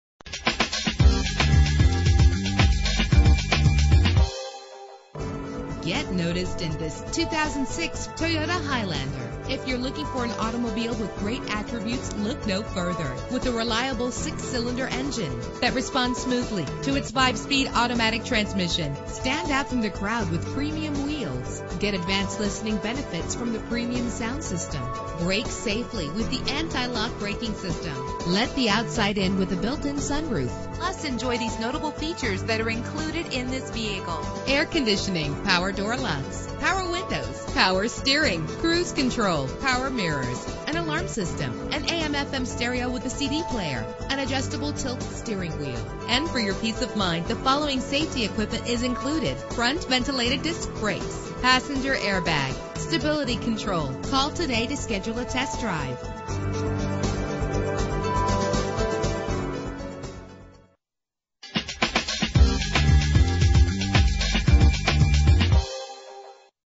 2006 Toyota Highlander - Feasterville PA
Year: 2006
Make: Toyota
Model: Highlander
Trim: Limited
Engine: 3.3 liter 6 cylinder 24 valve
Transmission: 5-Speed Automatic
Color: Gray
Mileage: 66298
Address:
117 Bustleton Pike